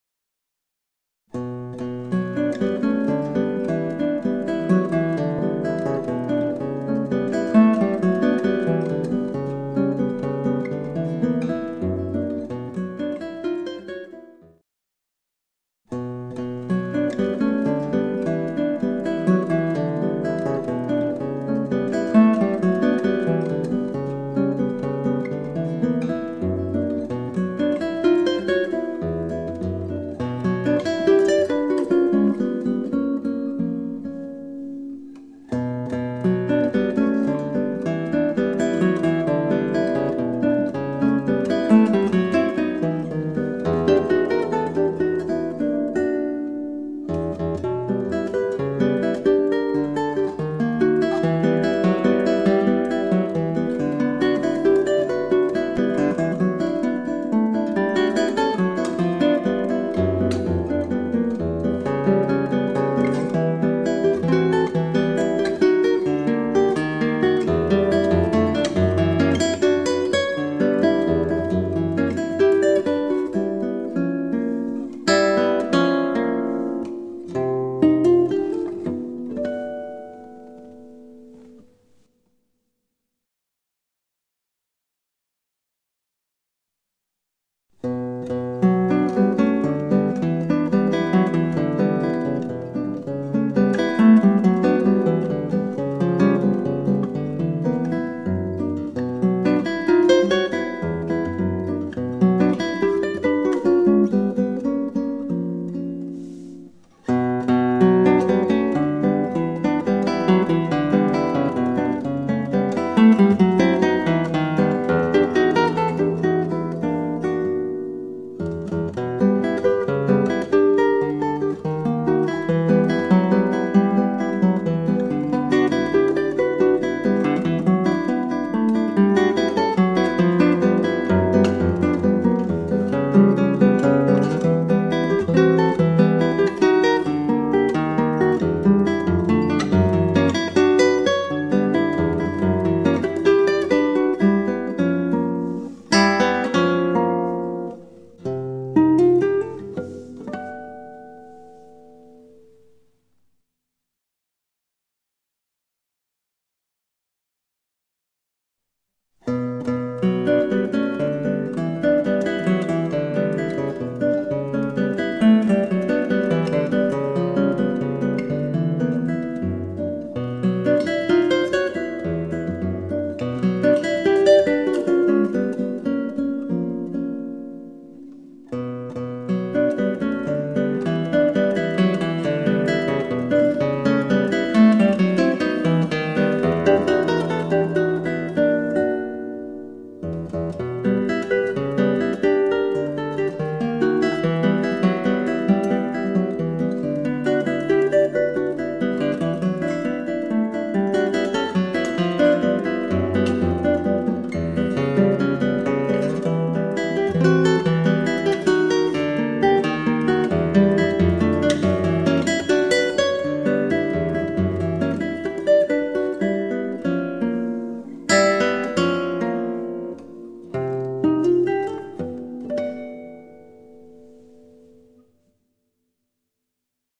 Tonewood comparison 3
A short video comparing 3 different tonewoods used for the back and sides.
Three recordings made with three guitars with very similar spruce soundboards, but using maple, Indian rosewood and Brazilian rosewood back and sides. The images in the videos aren't actually of the guitars used in the recordings, but are other examples of my guitars using similar construction and the same tonewoods as those in the recordings.
The recordings were made in the same room, with the same recording setup, and the guitars were played by my teacher Nick Fletcher.